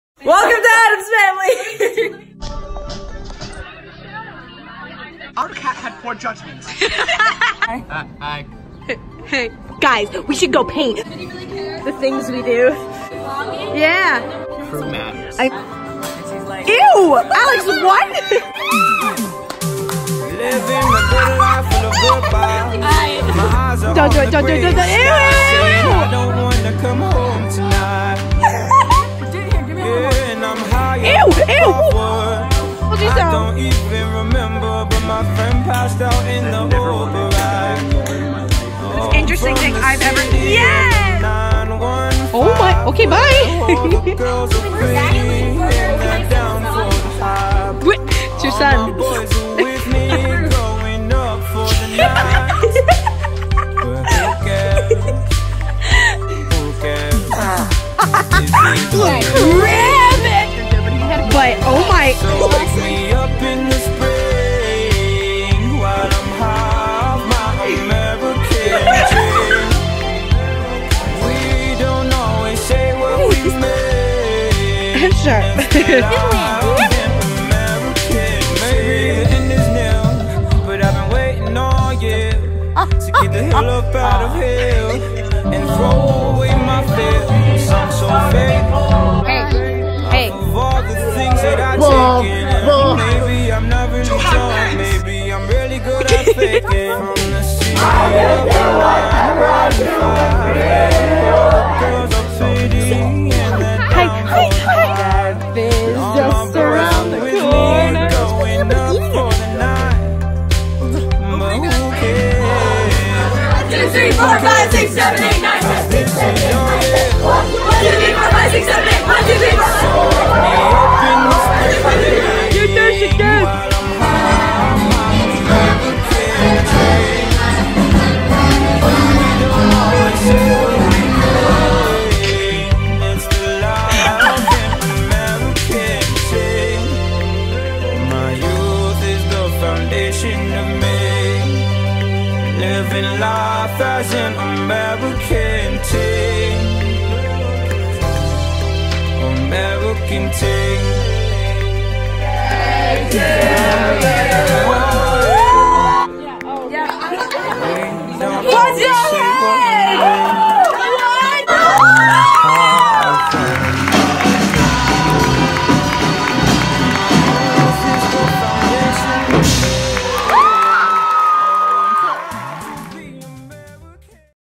Addams Family VHS 2019 - Thank You
Still so grateful for this remarkable cast, this unbelievable crew and the memories I will keep forever.

Edited by Gina Gagliano